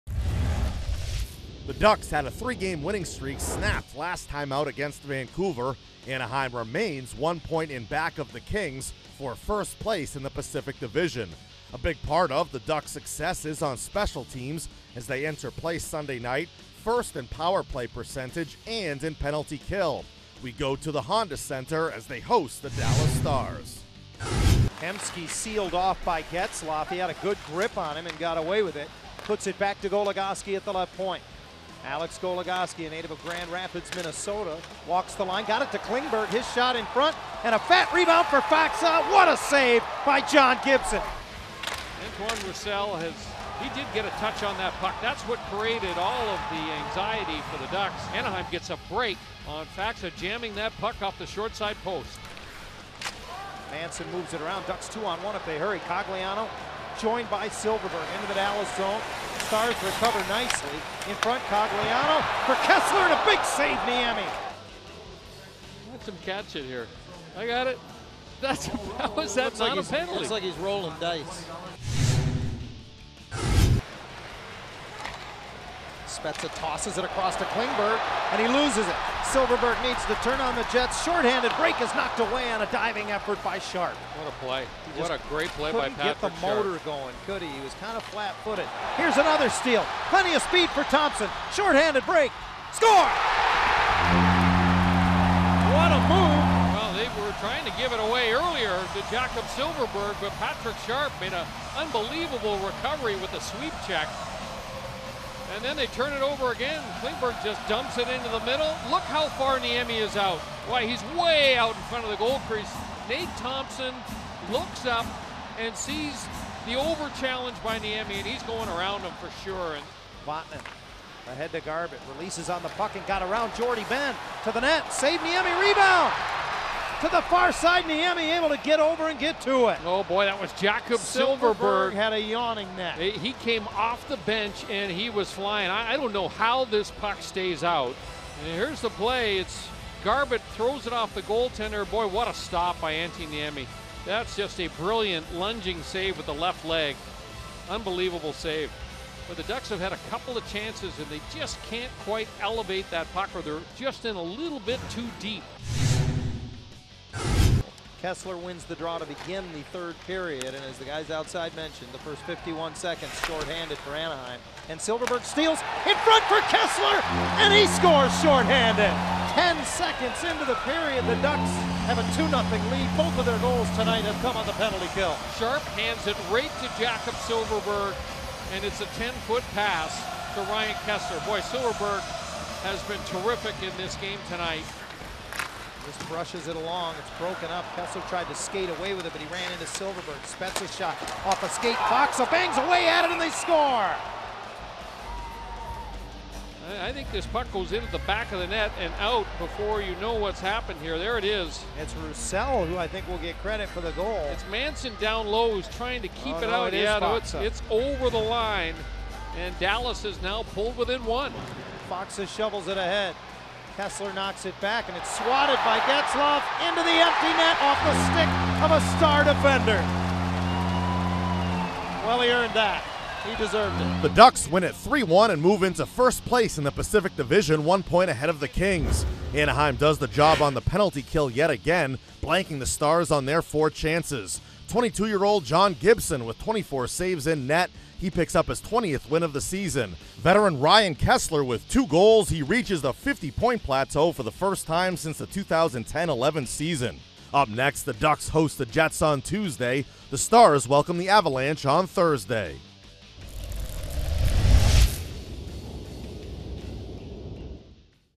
Ducks score two short-handed goals, beat Stars 3-1
The Ducks got shorthanded goals from Nate Thompson and Ryan Kesler and John Gibson made 24 saves to secure the win at home against the Stars